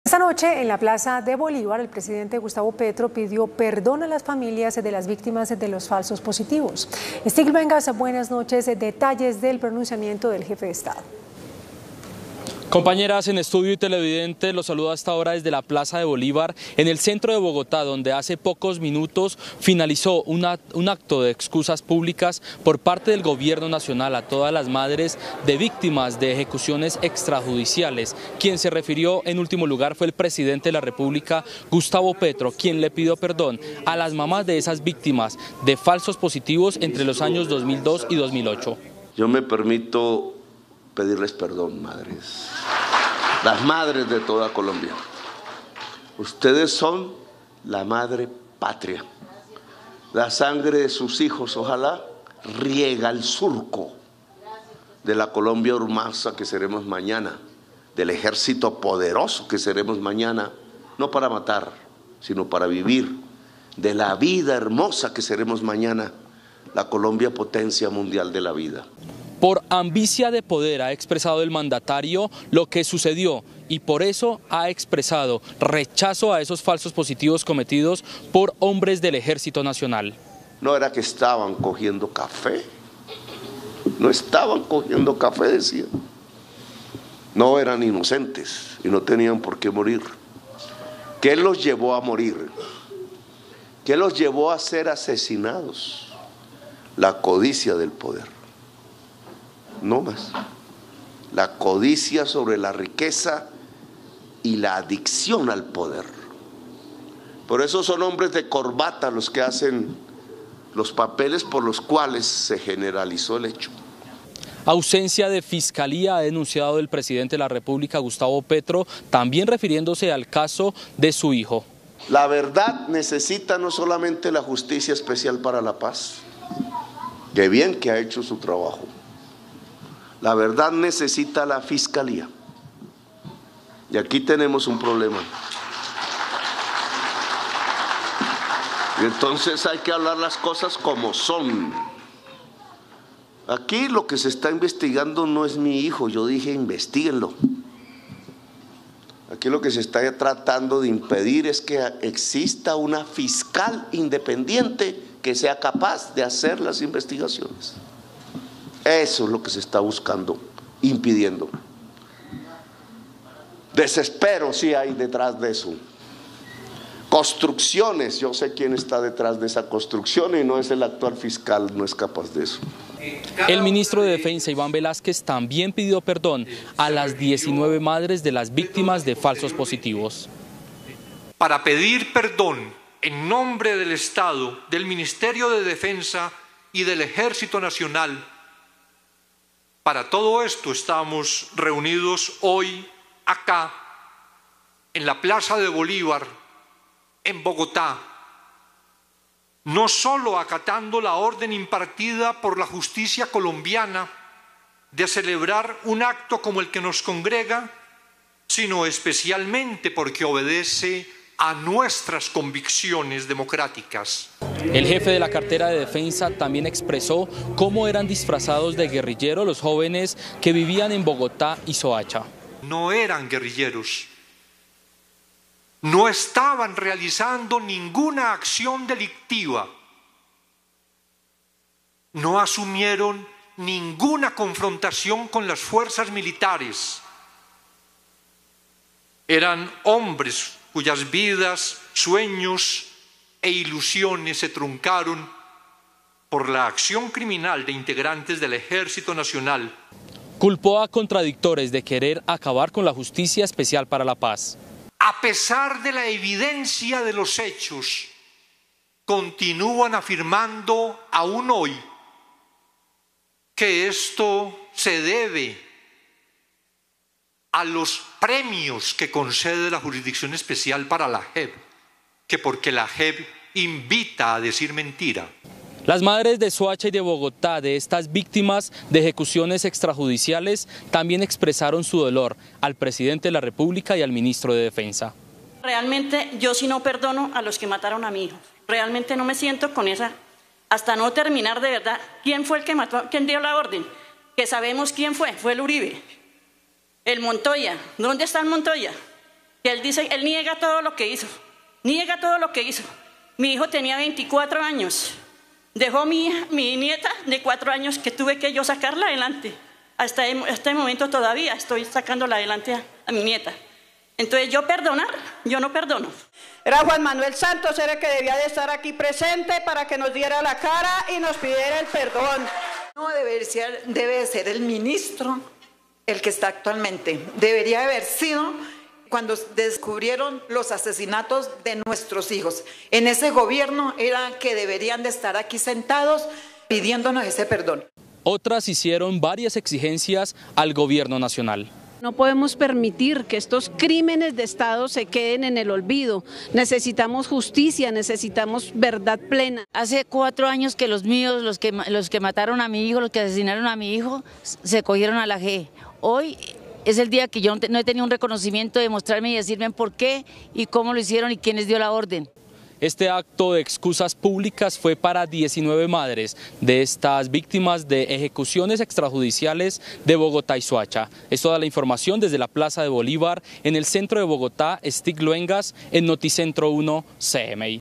El presidente Gustavo Petro pidió perdón por los falsos positivos | Canal 1
NoticiasEnVideo En la plaza de Bolívar, el presidente Gustavo Petro pidió perdón a las familias de las víctimas de los falsos positivos.